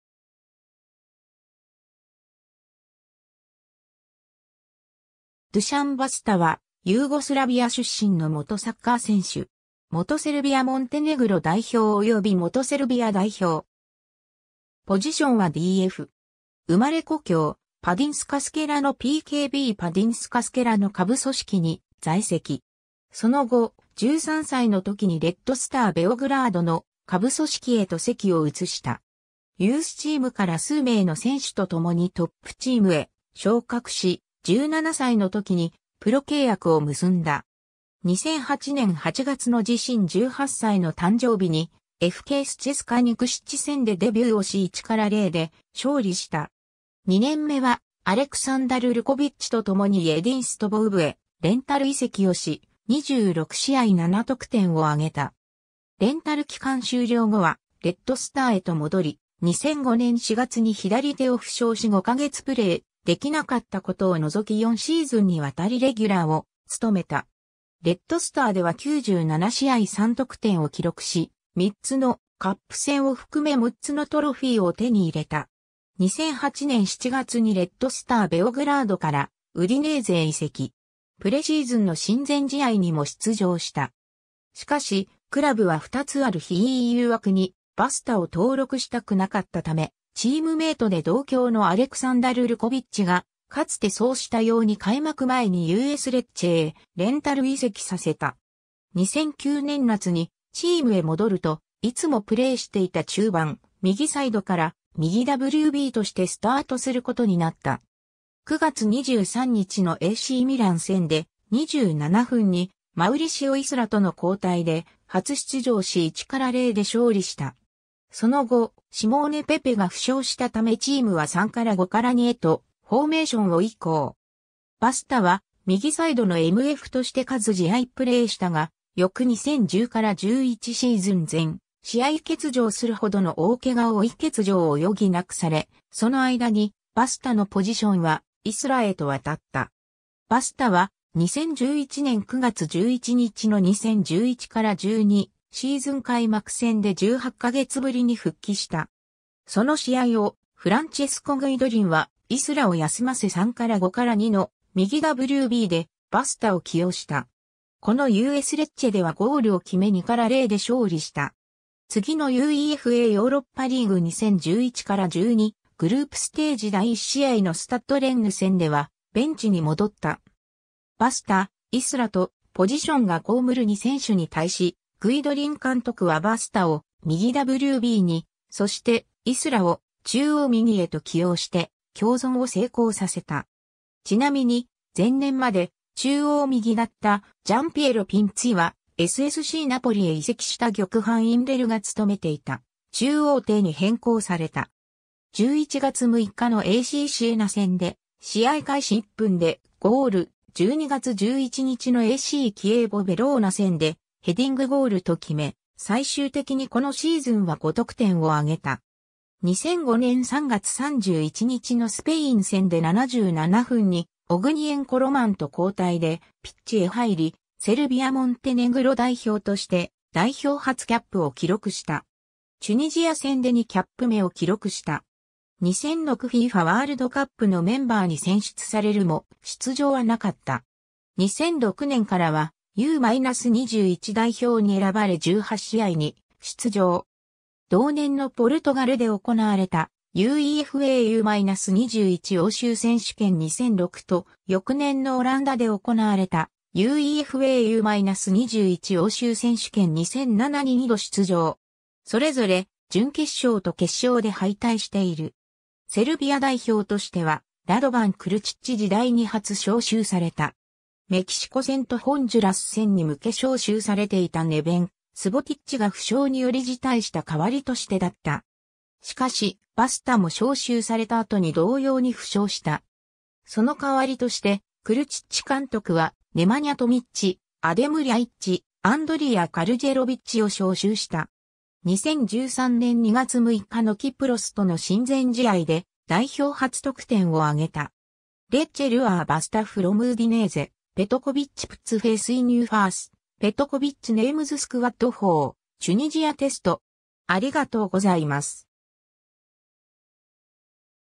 ドゥシャン・バスタ
セルビア・モンテネグロのサッカー選手
サッカーセルビア・モンテネグロ代表選手
レッドスター・ベオグラードの選手
FIFAワールドカップセルビア・モンテネグロ代表選手
ドゥシャン・バスタ（Dušan Basta, 1984年8月18日 - ）は、ユーゴスラビア出身の元サッカー選手。
元セルビア・モンテネグロ代表及び元セルビア代表。
 生まれ故郷パディンスカ・スケラのPKBパディンスカ・スケラの下部組織に在籍。
その後、13歳の時にレッドスター・ベオグラードの下部組織へと籍を移した。
 ユースチームから数名の選手と共にトップチームへ昇格し、17歳の時にプロ契約を結んだ。
2008年8月の自身18歳の誕生日に、FKスチェスカ・ニクシッチ戦でデビューをし1-0で勝利した。
2年目は、アレクサンダル・ルコヴィッチと共にイェディンストヴォ・ウブへレンタル移籍をし、26試合7得点を挙げた。
レンタル期間終了後はレッドスターへと戻り、2005年4月に左手を負傷し5ヶ月プレー出来なかったことを除き4シーズンに渡りレギュラーを務めた。
レッドスターでは97試合3得点を記録し、3つのカップ戦を含め6つのトロフィーを手に入れた。
 2008年7月にレッドスター・ベオグラードからウディネーゼへ移籍。
プレシーズンの親善試合にも出場した。
しかし、クラブは2つある非EU枠にバスタを登録したくなかったため、チームメイトで同郷のアレクサンダル・ルコヴィッチが、かつてそうしたように開幕前にUSレッチェへレンタル移籍させた。
 2009年夏にチームへ戻ると、いつもプレーしていた中盤右サイドから右WBとしてスタートすることになった。
9月23日のACミラン戦で、27分にマウリシオ・イスラとの交代で初出場し1-0で勝利した。
その後、シモーネ・ペペが負傷したためチームは3-5-2へとフォーメーションを以降。
バスタは右サイドのMFとして数試合プレーしたが、翌2010-11シーズン全試合欠場するほどの大怪我を負い欠場を余儀なくされ、その間にバスタのポジションはイスラへと渡った。
 バスタは、2011年9月11日の2011-12シーズン開幕戦で18ヶ月ぶりに復帰した。
その試合をフランチェスコ・グイドリンは、イスラを休ませ3-5-2の右WBでバスタを起用した。
このUSレッチェではゴールを決め2-0で勝利した。
次のUEFAヨーロッパリーグ 2011-12グループステージ第1試合のスタッド・レンヌ戦ではベンチに戻った。
バスタ, イスラとポジションが被る2選手に対し、グイドリン監督はバスタを右WBに、そしてイスラを中央右へと起用して共存を成功させた。
ちなみに、前年まで中央右だったジャンピエロ・ピンツィは、SSCナポリへ移籍したギョクハン・インレルが務めていた中央底に変更された。
11月6日のACシエナ戦で試合開始1分でゴール、12月11日のACキエーヴォ・ヴェローナ戦でヘディングゴールと決め、最終的にこのシーズンは5得点を挙げた。
 2005年3月31日のスペイン戦で77分にオグニェン・コロマンと交代でピッチへ入り、セルビア・モンテネグロ代表として、代表初キャップを記録した。
チュニジア戦で2キャップ目を記録した。
2006 FIFAワールドカップのメンバーに選出されるも出場はなかった。
 2006年からは、U-21代表に選ばれ18試合に出場。
同年のポルトガルで行われたUEFA U-21欧州選手権2006と、翌年のオランダで行われたUEFA U-21欧州選手権2007に2度出場。
それぞれ準決勝と決勝で敗退している。
 セルビア代表としては、ラドヴァン・クルチッチ時代に初招集された。
メキシコ戦とホンジュラス戦に向け招集されていたネヴェン・スボティッチが負傷により辞退した代わりとしてだった。
しかし、バスタも招集された後に同様に負傷した。
その代わりとしてクルチッチ監督は、ネマニャ・トミッチ, アデム・リャイッチ, アンドリヤ・カルジェロヴィッチを招集した。
 2013年2月6日のキプロスとの親善試合で代表初得点を挙げた。
 ^ "Lecce lure Basta from Udinese" ^ "Petkovic puts faith in new faces" ^ "Petković names s...